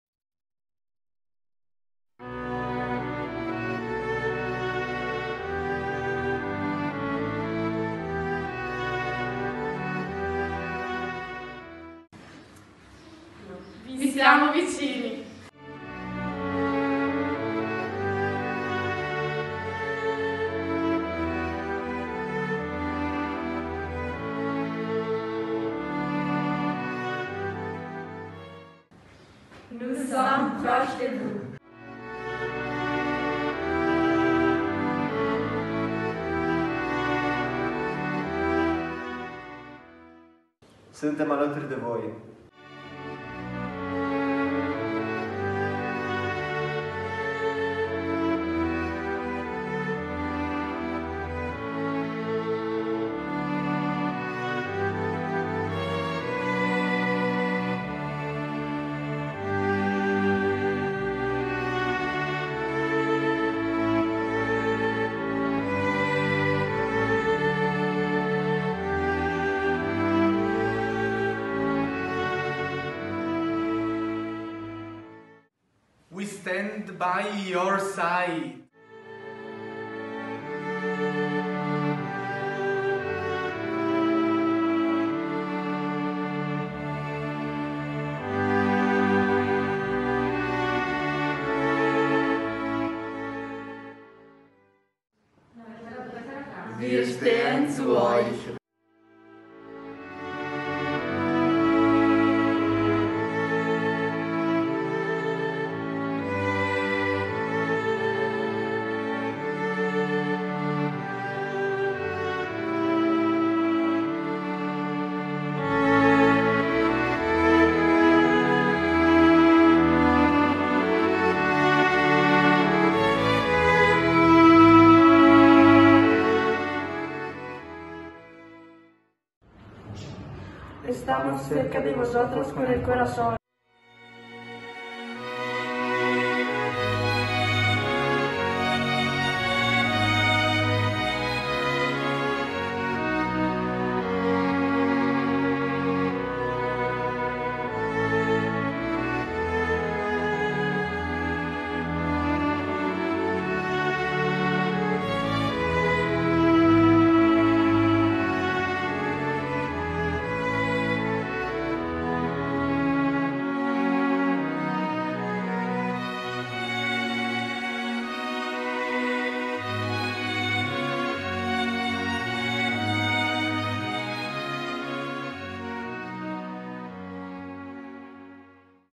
Give Peace a Chance #102 3DLIN
A Gambara Limited Series – Give Peace a Chance – 3DLIN (2021-22) con la Prof.ssa Airò
In seguito all’invasione dell’Ucraina, gli studenti e le studentesse di 3DLIN hanno voluto dare la loro risposta alla guerra e invitare tutti a contribuire alla pace.